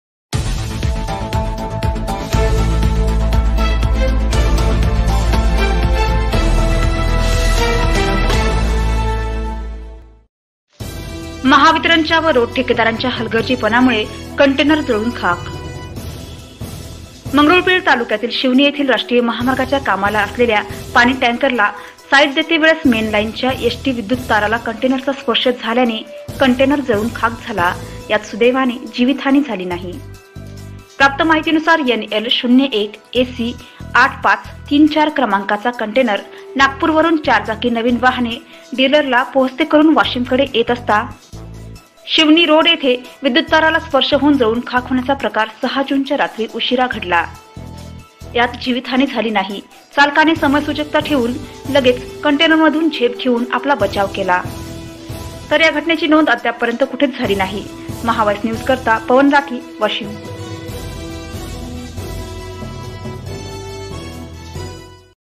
महावितरण च्या व रोड ठेकेदाराच्या हलगर्जीपणा मुळे कंटेनर जळून खाक...
मंगरुळपीर तालुक्यातील शिवणी येथे राष्ट्रीय महामार्गाचे कामाच्या पाणी टँकरला साईड देतेवेळी मेन लाईनच्या HT विद्युत ताराला कंटेनरचा स्पर्श झाल्याने कंटेनर जळुन खाक यात सुदेवाने जिवीतहानी नाही

व्हिओ
प्राप्त माहीती नुसार NL 01-AC-8534 क्रमांकाचे कंटेनर नागपूर वरून चारचाकी नवीन वाहने डिलरला पोहचते करुन वाशिम कडे येत असता.शिवनी(रोड) येथे विद्युत ताराला स्पर्श होऊन जळुन खाक होण्याचा प्रकार 6 जुन च्या रात्री उशिरा घडला यात जिवीत हाणी नाही चालकाने समयसूचकता ठेवुन लगेच कंटेनर मधुन झेप घेवुन आपला बचाव केला.तर या घटनेची नोंद अद्याप पर्यंत कुठेच झालेली नाही. महाव्हाइस न्युज  करीता पवन राठी वाशिम